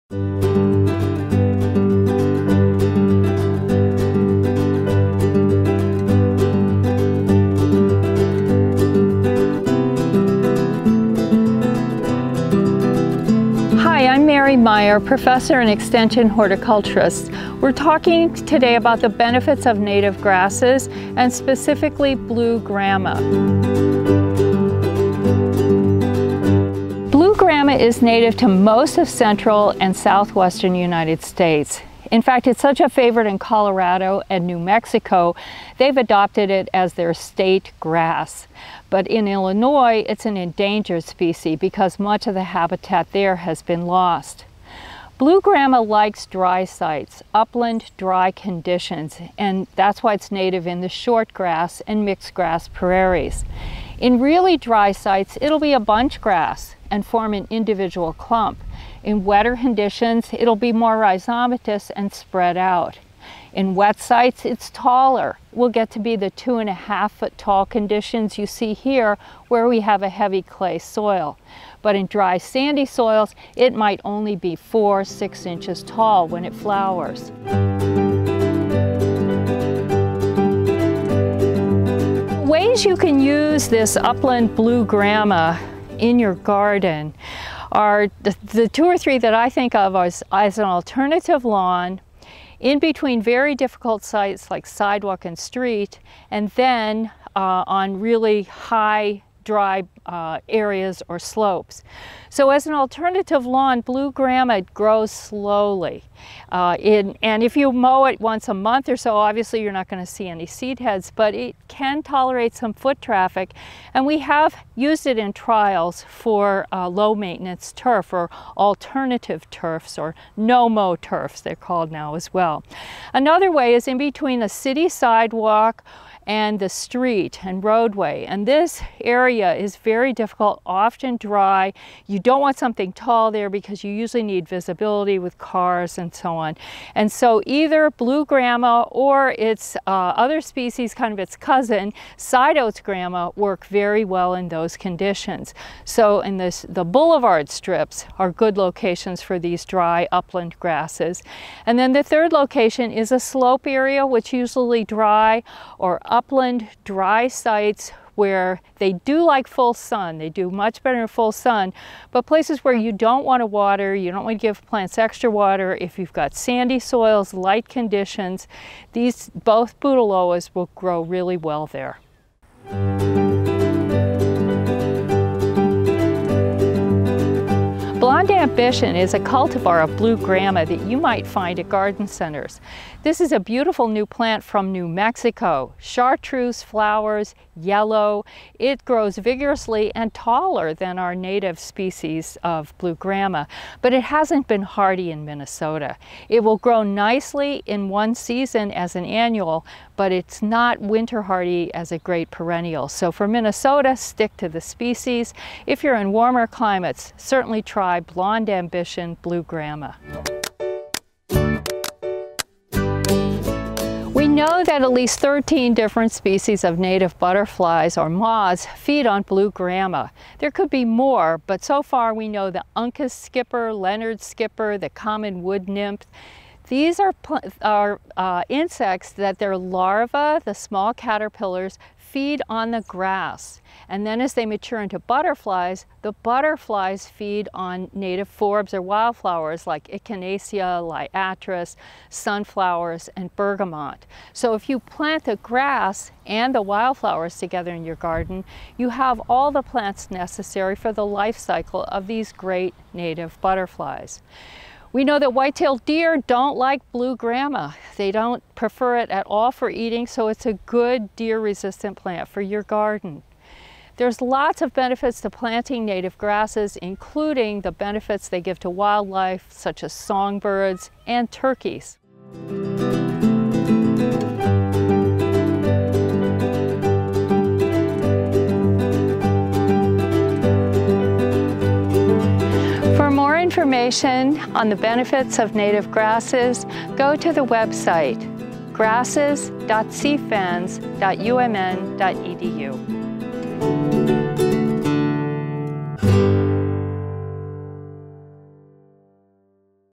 Benefits of Native Grasses: Blue Grama
Native grasses like blue grama have many benefits for gardens in Minnesota. UMN Extension Professor and Horticulturist Mary Meyer explains how this grass can be used on slopes, in rain gardens and in dry locations such as street boulevard gardens. Endangered species of butterflies in Minnesota feed on blue grama. One of a 3-part video series on the benefits of native grasses.